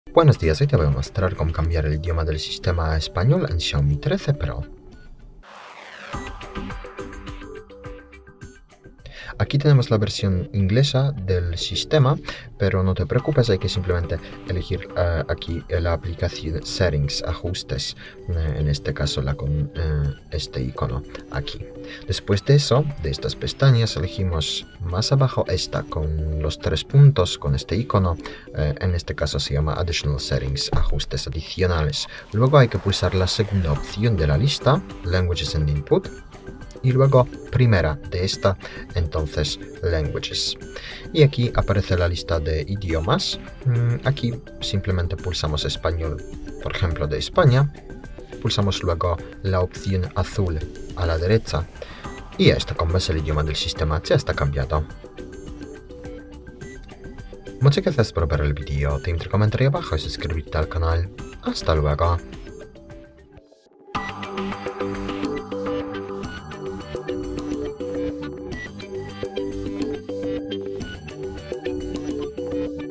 Commo cambiar el idioma a español en Xiaomi 13 Pro
¡Hola a todos! En nuestro tutorial vamos a explicar cómo cambiar el idioma en Xiaomi 13 Pro y cómo activar el español en Xiaomi 13 Pro. Para aprender a cambiar el idioma de inglés al español en Xiaomi 13 Pro quédate con nosotros y sigue el vídeo.